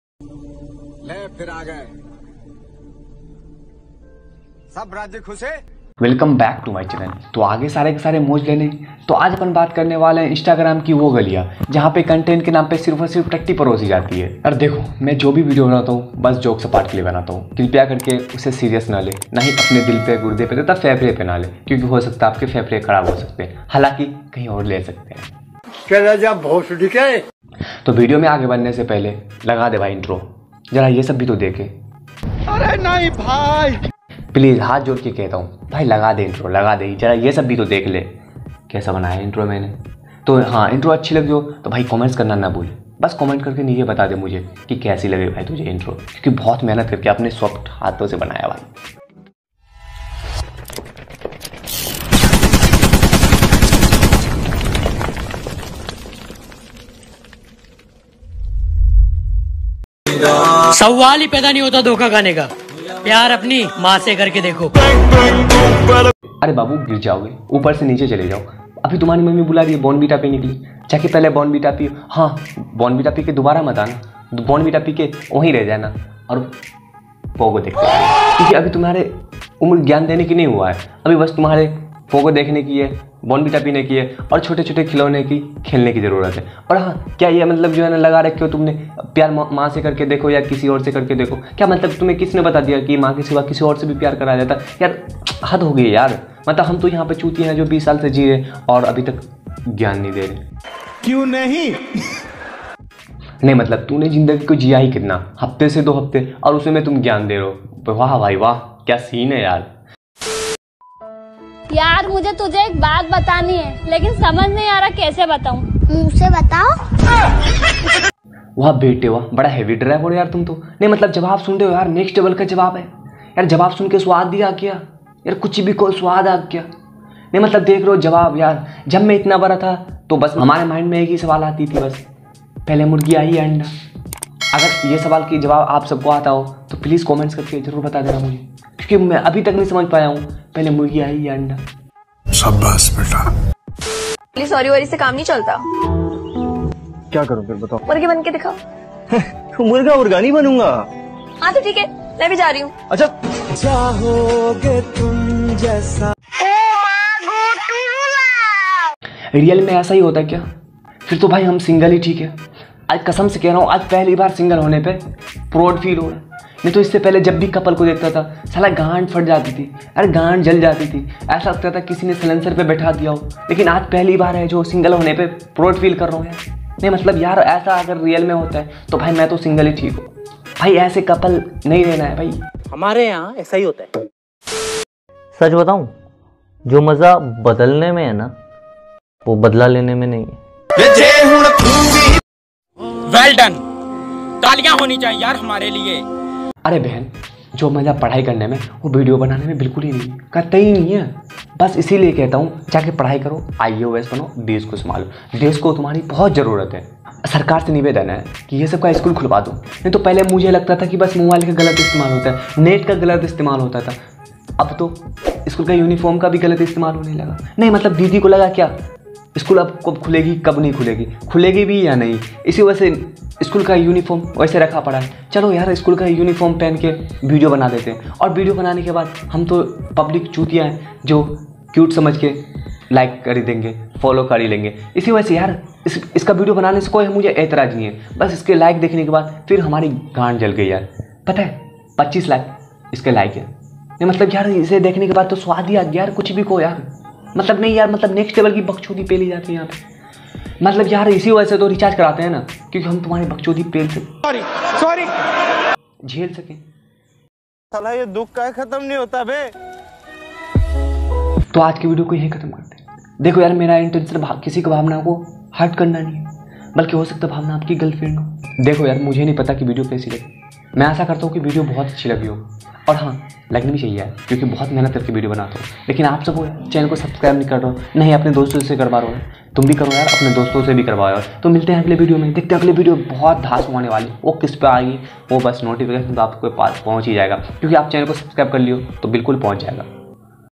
Instagram reels Roast | crazy boi
Instagram reels Roast. reels Roast. aj ke video me Instagram reels ki backchodi ko roast kiya hu plz apna pyar banae rakhe